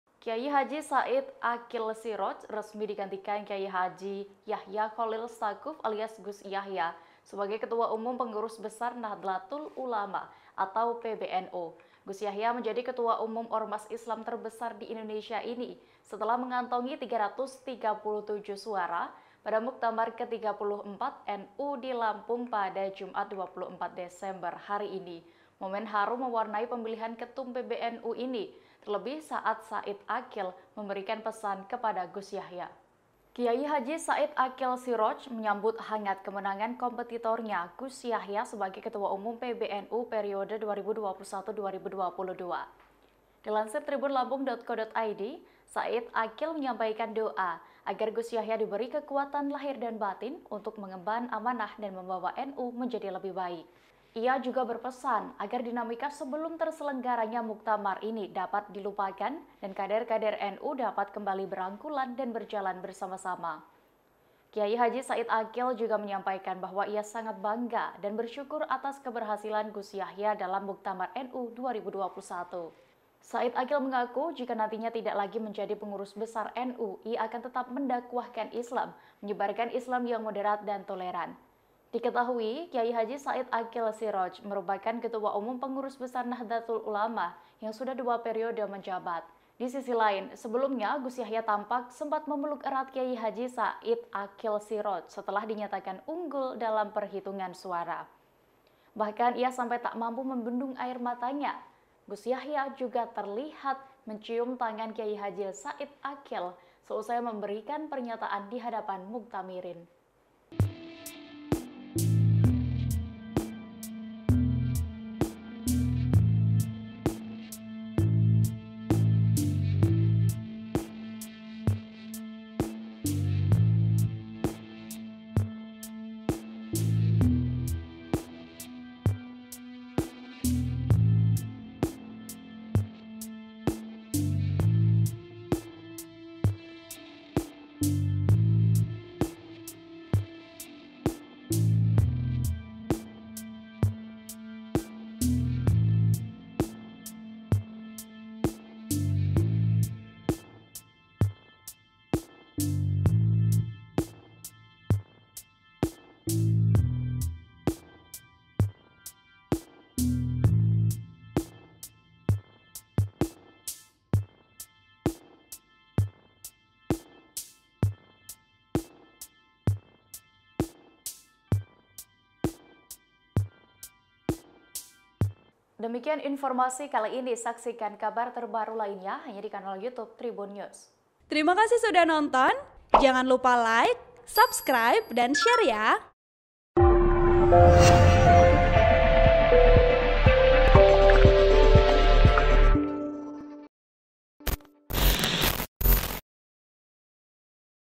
Pesan Haru Said Aqil Kepada Ketua Umum PBNU yang Gantikannya, Gus Yahya Tak Kuasa Bendung Air Mata
TRIBUN-VIDEO.COM - KH Said Aqil Siradj resmi digantikan KH Yahya Cholil Staquf alias Gus Yahya sebagai Ketua Umum Pengurus Besar Nahdlatul Ulama (PBNU).

Gus Yahya menjadi ketua umum Ormas Islam terbesar di Indonesia itu setelah mengantongi 337 suara pada Muktamar Ke-34 NU di Lampung, Jumat (24/12/2021).

Momen haru mewarnai pemilihan Ketum PBNU itu, terlebih saat Said Aqil memberikan pesan kepada Gus Yahya.

KH Said Aqil Siraj menyambut hangat kemenangan kompetitornya Gus Yahya sebagai ketua umum PBNU periode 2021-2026.

Dilansir TribunLampung, Said Aqil menyampaikan doa agar Gus Yahya diberi kekuatan lahir batin untuk mengemban amanah dan membawa NU menjadi lebih baik.

"Mudah mudahan Allah memberi kekuatan lahir batin ke beliau sehingga mampu memimpin PBNU ke depan lebih baik lagi," ujar Said Aqil Siroj di Gedung Serbaguna Universitas Lampung, Bandar Lampung, Jumat (24/12/2021).

"Gus Yahya adalah cicit dari gurunya ayah saya, itu kiainya kakek nomor 4, ayah saya belajar ngaji khatam alfiah bimbingan kakek keempat buyutnya Gus Yahya," terangnya.

Ia juga berpesan agar dinamika sebelum terselenggaranya Muktamar dapat dilupakan dan kader-kader NU dapat kembali berangkulan dan berjalan bersama-sama.

"Kita lupakan apa yang kemarin terjadi kita lupakan kita bergandengan tangan bersama-sama membesarkan NU," pungkasnya.

KH Said Aqil juga menyampaikan bahwa ia sangat bangga dan bersyukur atas keberhasilan Gus Yahya dalam Muktamar NU 2021.

"Saya juga bersyukur Muktamar NU berjalan dengan baik, walaupun sempat agak panas, tapi alhamdulillah selesai dengan damai. Sekarang, kita lupakan apa yang sudah terjadi kemarin, kita bergandengan tangan bersama-sama membesarkan NU," ucap Said Aqil, Jumat (24/12/2021).

Said Aqil mengaku, jika nantinya tidak lagi menjadi pengurus besar NU, ia akan tetap mendakwahkan Islam, menyebarkan Islam yang moderat dan toleran.

Diketahui KH Said Aqil Siradj merupakan Ketua Umum Pengurus Besar Nahdlatul Ulama (PBNU) yang sudah dua periode menjabat.

"Semampu saya," ucap Said Aqil.

Sebelumnya Gus Yahya tampak sempat memeluk erat KH Said Aqil Siradj setelah dinyatakan unggul dalam penghitungan suara.

Bahkan ia sampai tak mampu membendung air matanya.

Gus Yahya juga terlihat cium tangan KH Said Aqil Siradj seusai memberikan pernyataan di hadapan muktamirin.(Tribun-video.com/TribunLampung.co.id)

Artikel ini telah tayang di TribunLampung.co.id dengan judul Ketum PBNU Gus Yahya Cium Tangan Said Aqil Siradj, 'Lupakan yang Terjadi Kemarin',